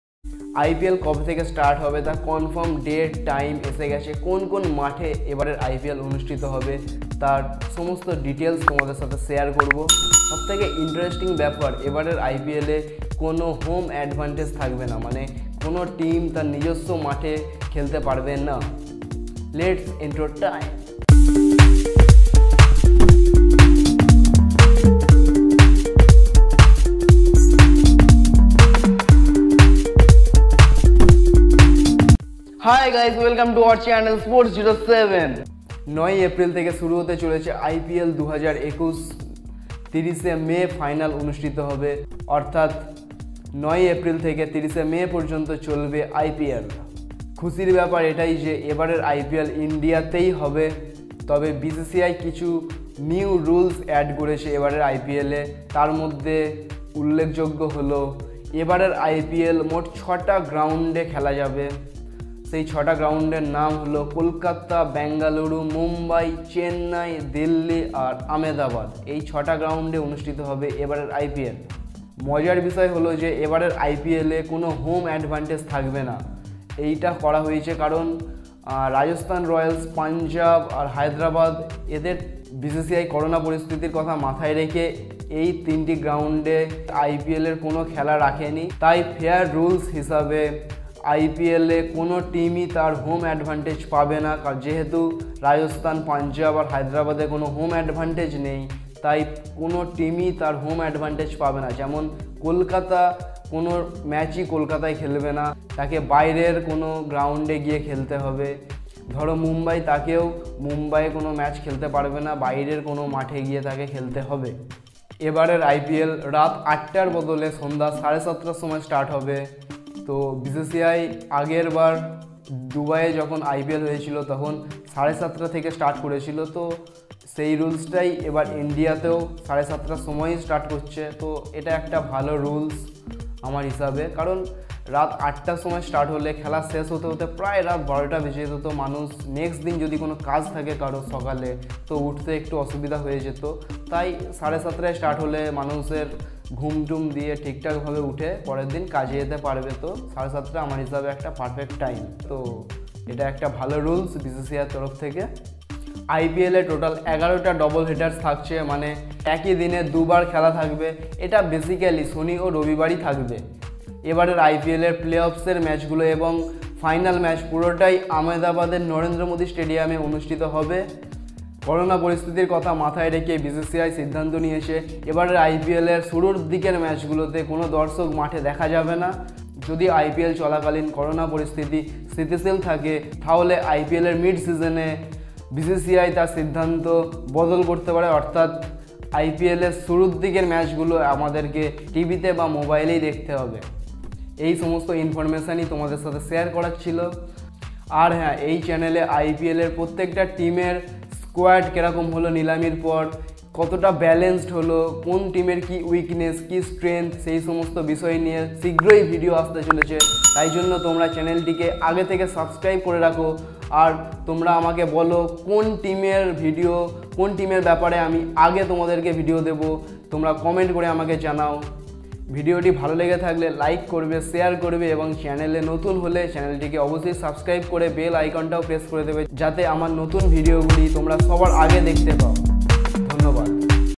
IPL 2021 – Confirm Date, Time, Venue & New Rules😱 |Sports 07| BCCI Announced IPL 2021 Full Schedule.
IPL 2021 is here guys. The full schedule has been announced, and that’s what we are discussing on this special topics. Today, I talk about IPL’s date, time, venue and some new rules from BCCI. Let’s see friends…